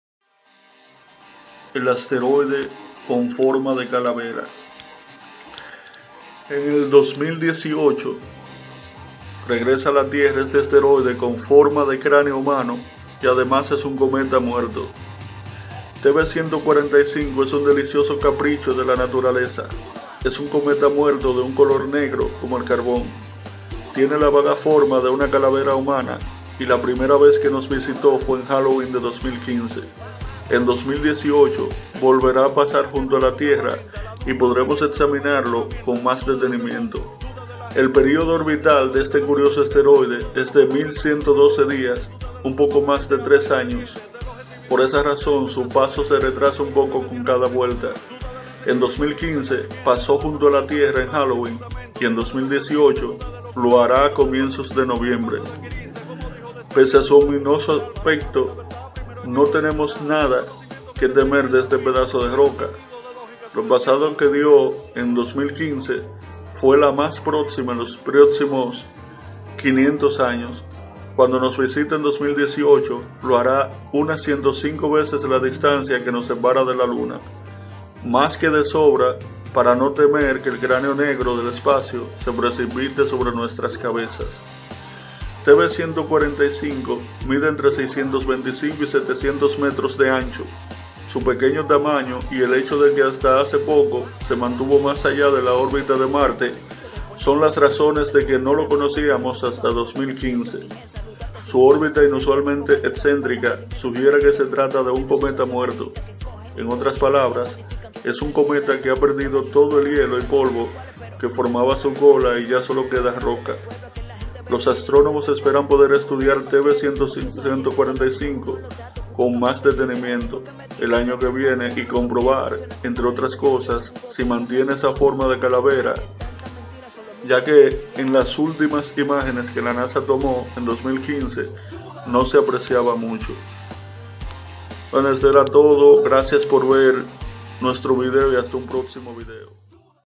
El asteroide con forma de cráneo o calavera TB145
En 2018 regresa a la Tierra este asteroide con forma de cráneo humano que, además, es un cometa muerto

TB145 es un delicioso capricho de la naturaleza. Es un cometa muerto de un color negro como el carbón, tiene la vaga forma de una calavera humana y la primera vez que nos visitó fue en Halloween de 2015. En 2018 volverá a pasar junto a la Tierra y podremos examinarlo con más detenimiento
El período orbital de este curioso asteroide es de 1.112 días, un poco más de tres años. Por esa razón su paso se retrasa un poco con cada vuelta. En 2015 pasó junto a la Tierra en Halloween. En 2018 lo hará a comienzos de noviembre.
Pese a su ominoso aspecto, no tenemos nada que temer de este pedazo de roca. La pasada que dió en 2015 fue la más próxima en los próximos 500 años. Cuando nos visite en 2018 lo hará a unas 105 veces la distancia que nos separa de la Luna, más que de sobra como para no temer que el cráneo negro del espacio se precipite sobre nuestras cabezas.
TB145 mide entre 625 y 700 metros de ancho. Su pequeño tamaño y el hecho de que hasta hace poco se mantuvo más allá de la órbita de Marte son las razones de que no lo conociéramos hasta 2015. Su órbita inusualmente excéntrica sugiere que se trata de un cometa muerto. En otras palabras, es un cometa que ha perdido todo el hielo y polvo que formaba su cola y ya solo queda roca. Los astrónomos esperan poder estudiar TB145con más detenimiento el año que viene y comprobar, entre otras cosas, si mantiene esa forma de calavera. En las últimas imágenes que la NASA tomó en 2015 no se apreciaba mucho.